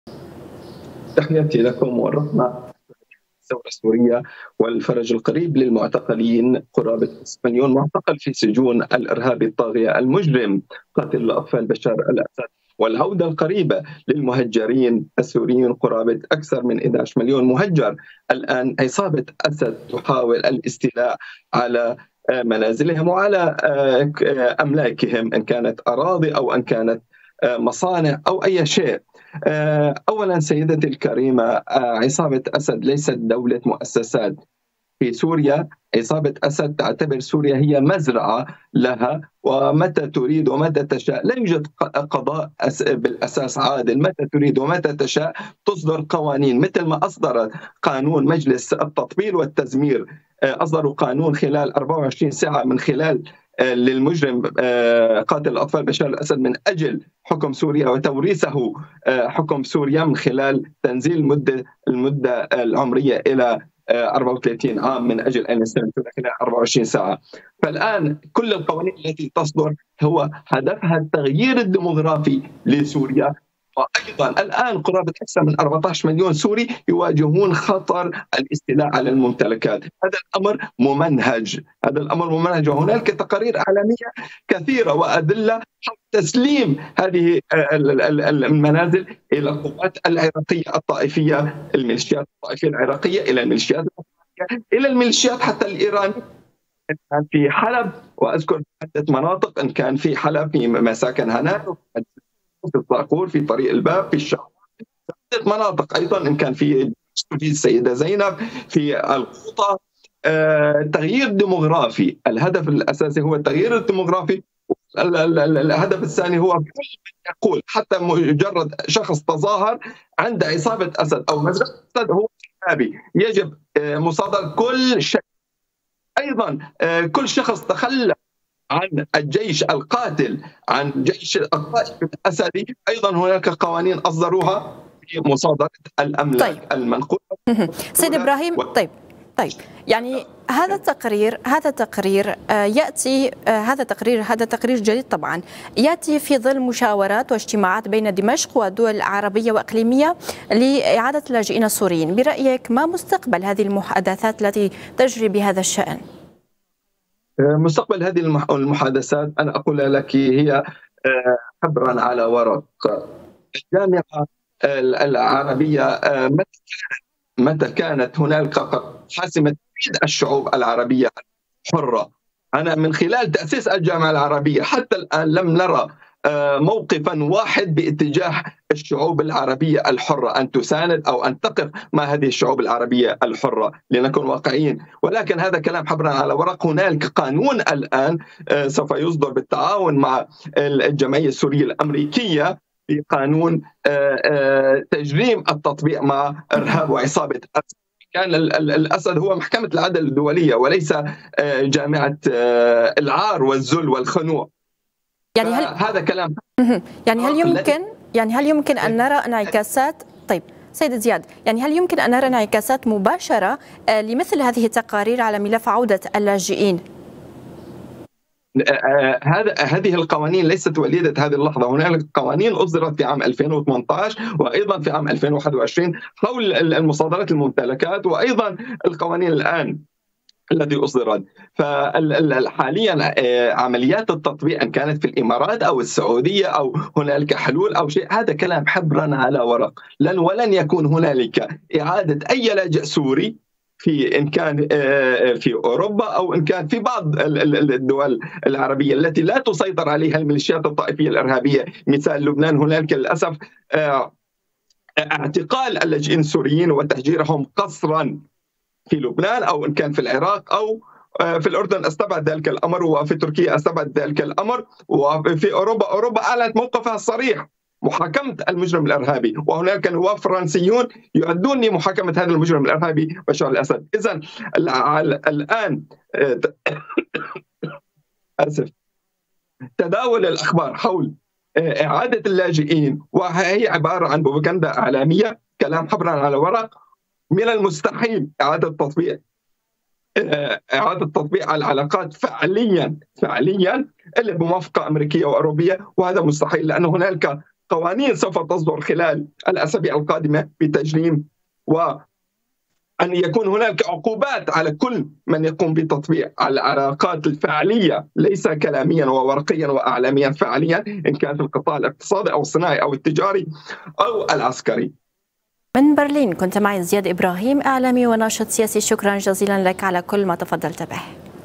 زياد إبراهيم_ تقرير يسلط الضوء على قوانين استخدمتها الحكومة السورية للاستيلاء على ممتلكات اللاجئين
سوريا
تقرير يسلط الضوء على قوانين استخدمتها الحكومة للاستيلاء على ممتلكات اللاجئين
برلين
زياد إبراهيم - إعلامي وناشط سياسي
البث الحي / البث المباشر الأعلى مشاهدة

تابعوا قناة اليوم على التردد:
12687_27500 (H) _NILESAT